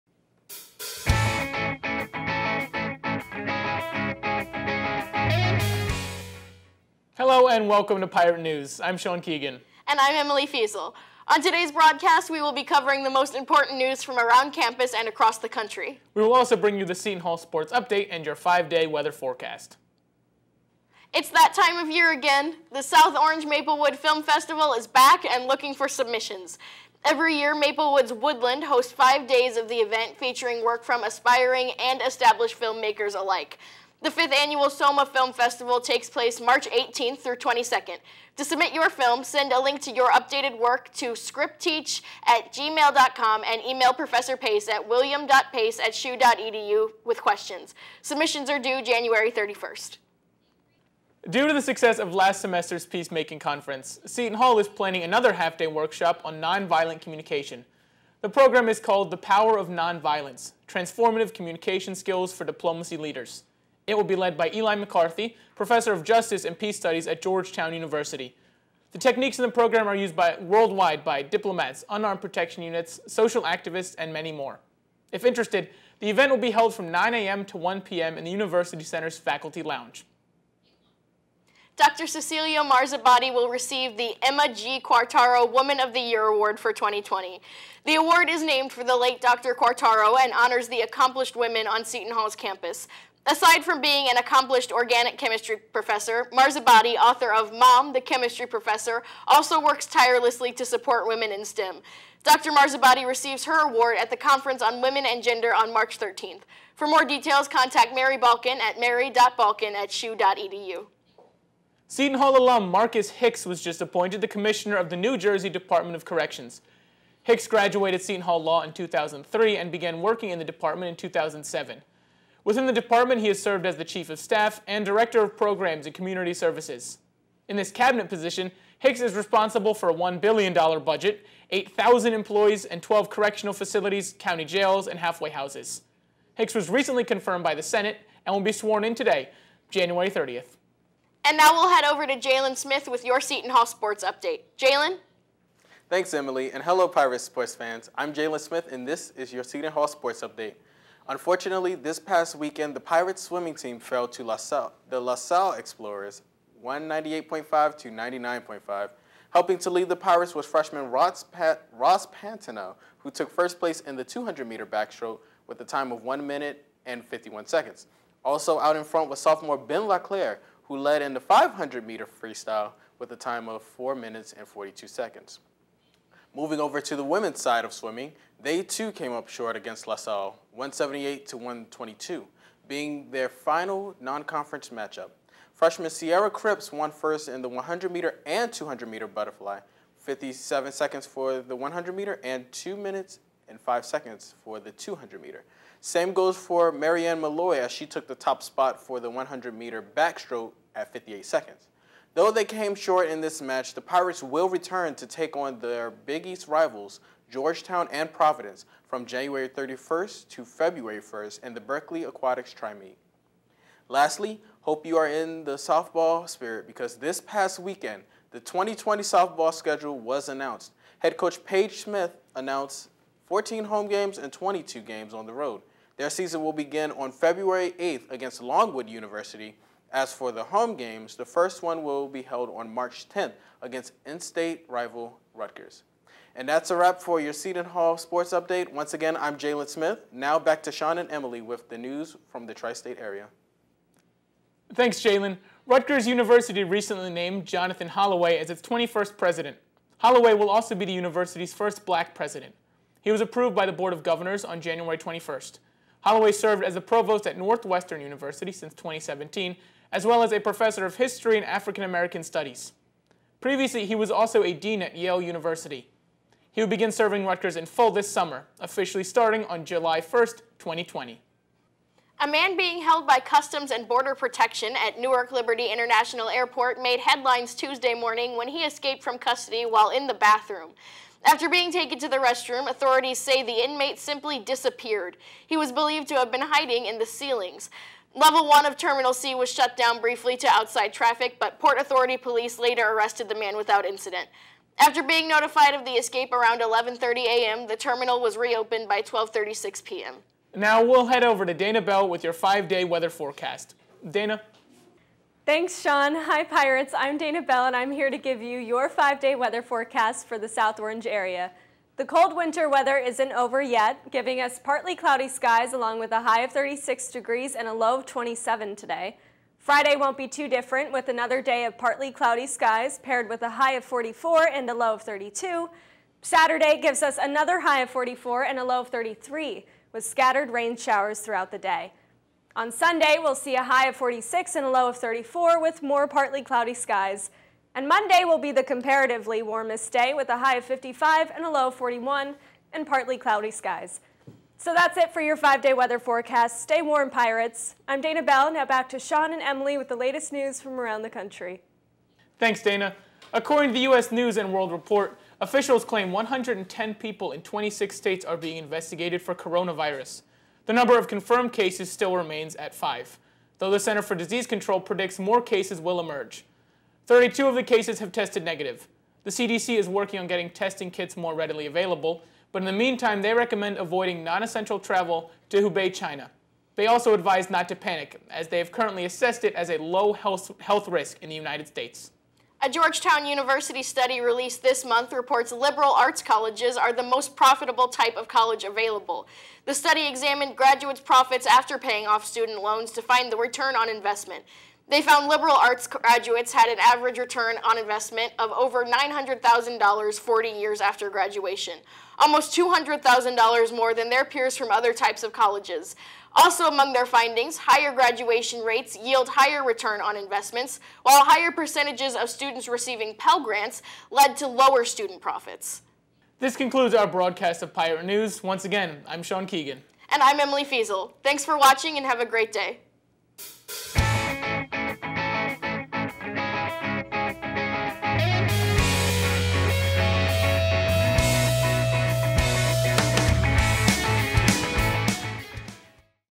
Pirate News - January 30th, 2020
Sean Keegan and Emily Feazel bring you the latest in Seton Hall, local, and national news. Jaylyn Smith gives you the latest updates for Seton Hall sports and Dana Bell has your 5-day weather forecast for the South Orange area.

Follow Seton Hall Pirate TV on Facebook, Twitter, Instagram, and Wordpress for all the Seton Hall news you need to know!